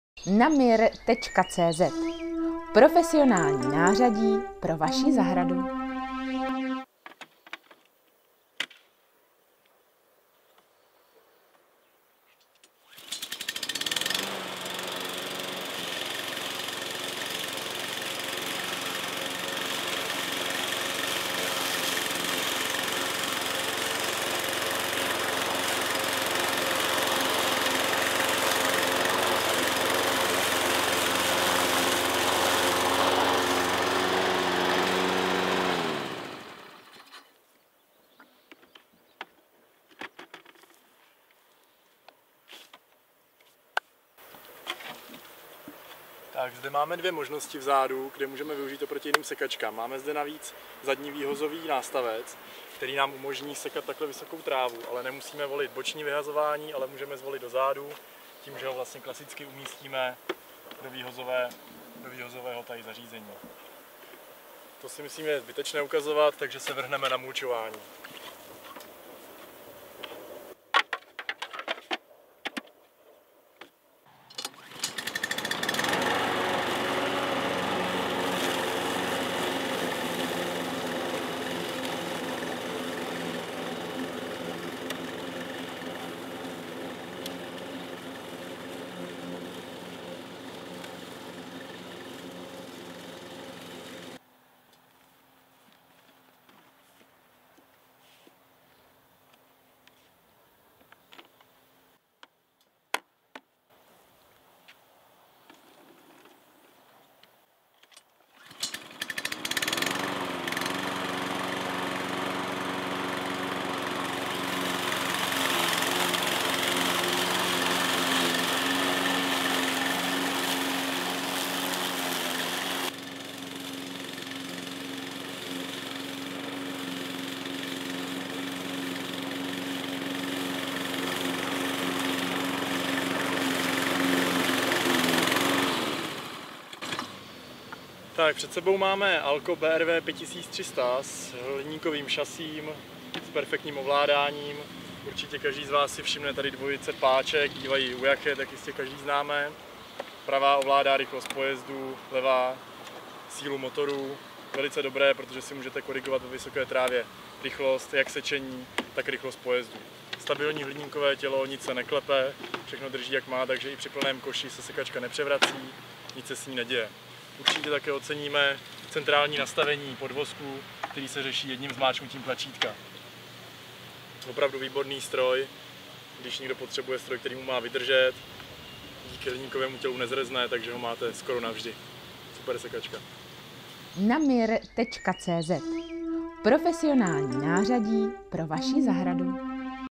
Test sekačky PowerLine 5300 BRV ALUMINIUM AL-KO - 1. místo, 97%
Průběh testu sekačky PowerLine 5300 BRV ALUMINIUM AL-KO, která dosáhla celkového hodnocení 97% a umístila se na 1. místě. Rozsáhlejší slovní hodnocení naleznete ve videu.